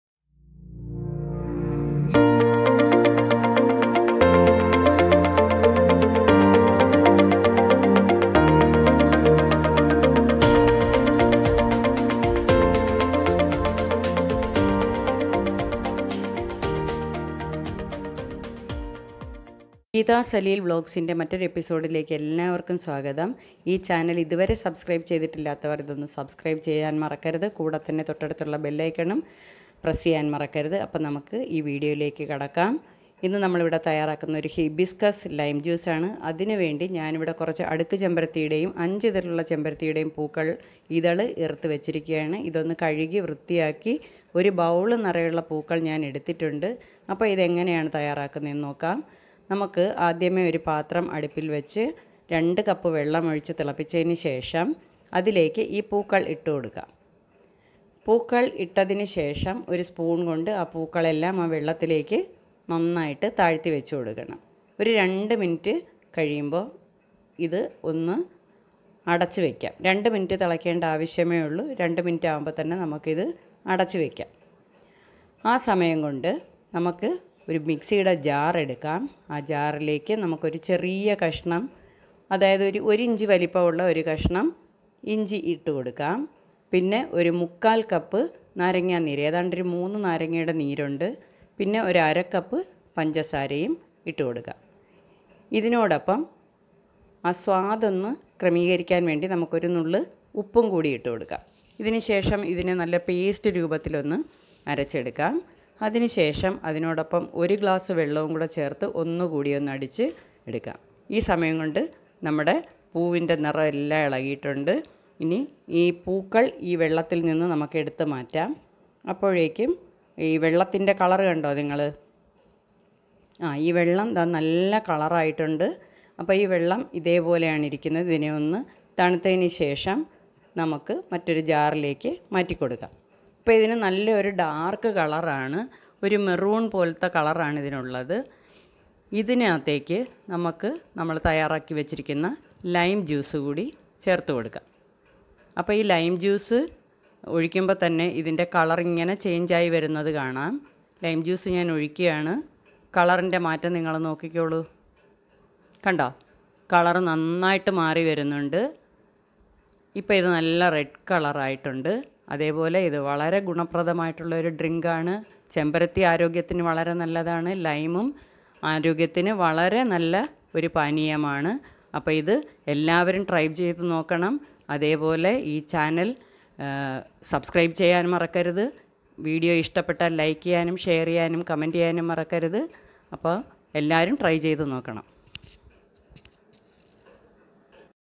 HIBISCUS LIME JUICE || Geetha Salleel Vlogs - 3 || കൊതിയൂറും നിറമേറും ചെമ്പരത്തി ജ്യൂസ്
Hibiscus Lime Juice -

Ingredients :
Hibiscus flowers - 8 to 10

Hibiscus flower is of great medicinal values. It is good for our body system and metabolism.
Here I am presenting before you a colourful and healthy drink for this summer.

I hope you all will try making this at home ❤️